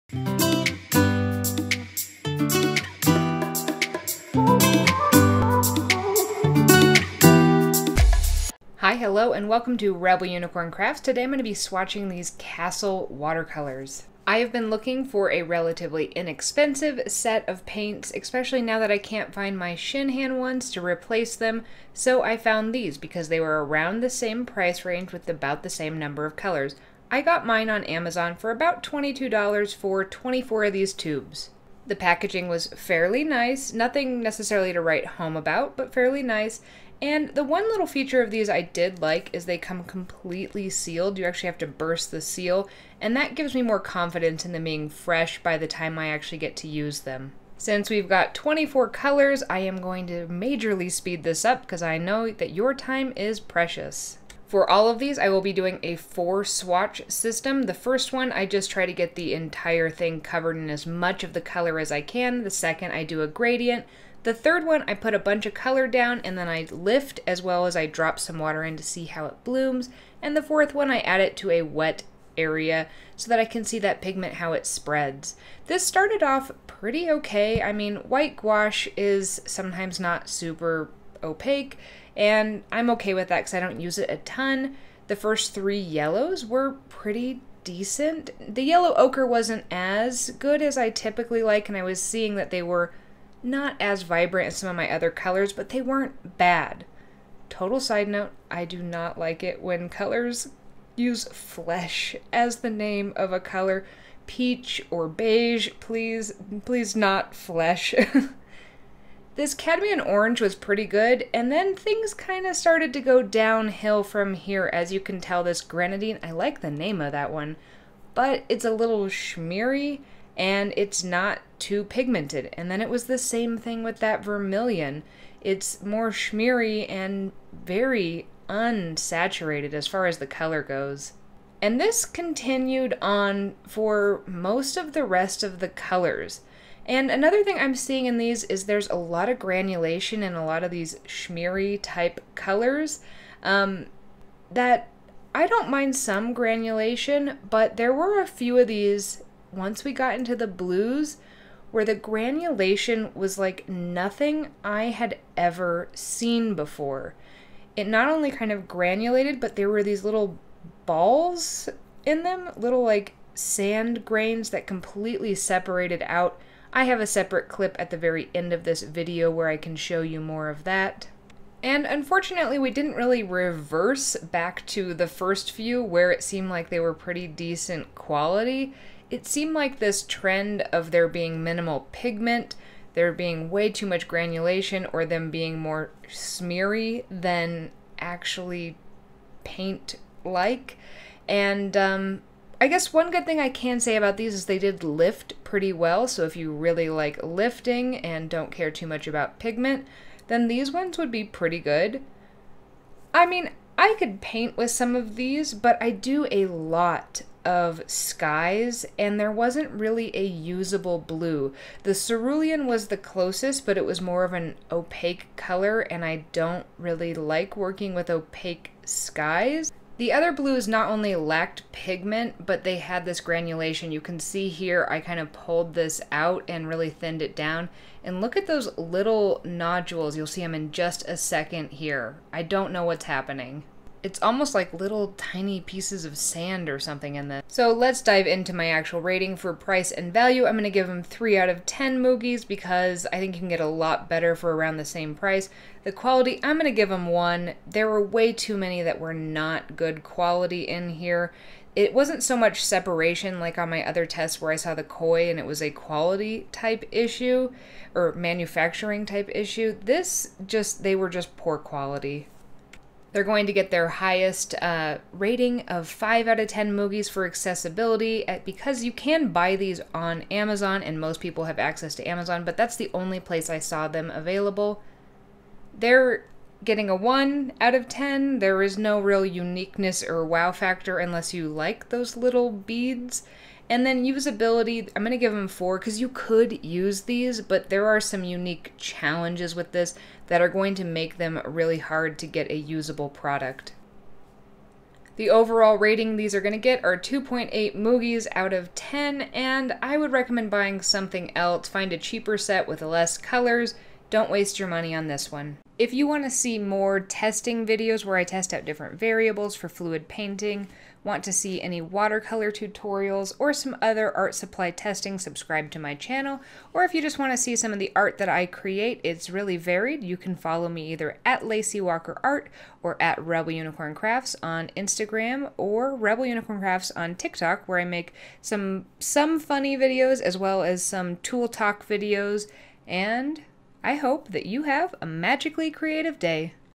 Swatching Castle Watercolors - What is happening?!
I swatched Castle Watercolor Tubes.  Watch along to see how they stack up!